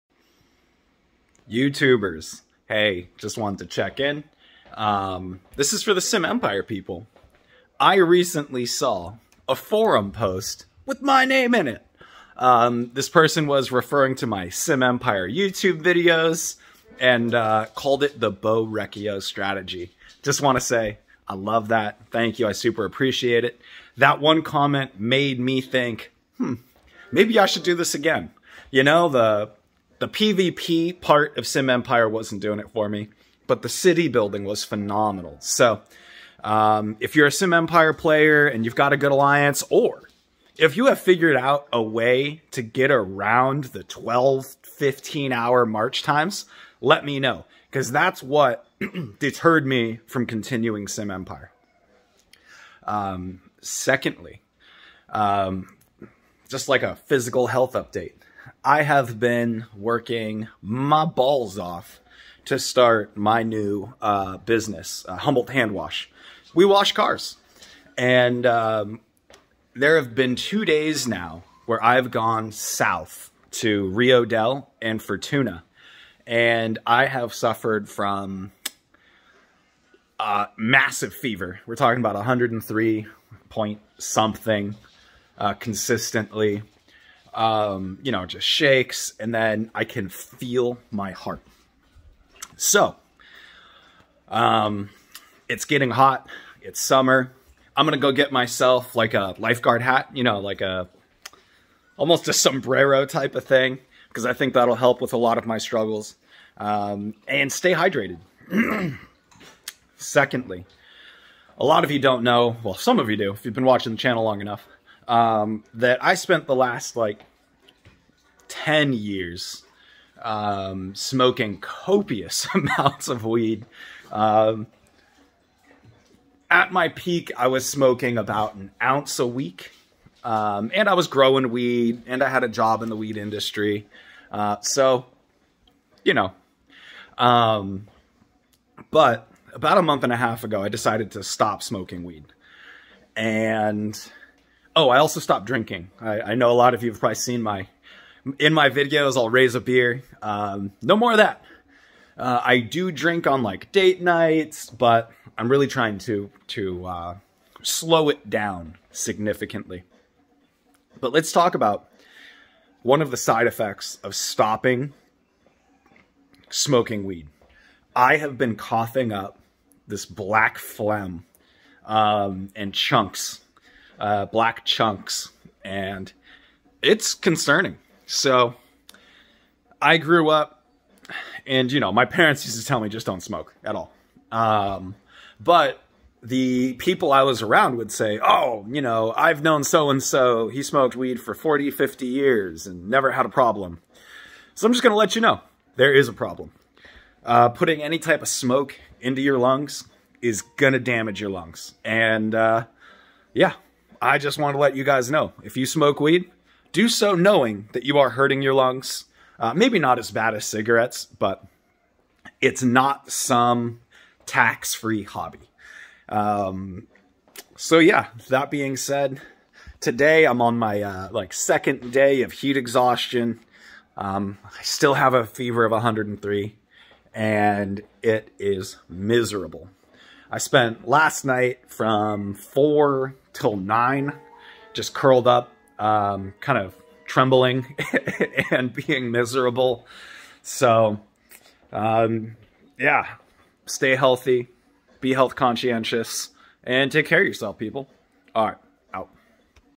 Sim Empire, Health Update (Heat Exhaustion, Quitting Weed and Drinking Alcohol)
Thanks to the guy who mentioned me on Sim Empire Forums! Also, I'm no longer smoking weed or Drinking (as much) and also Dealing with Heat Exhaustion from working in the heat.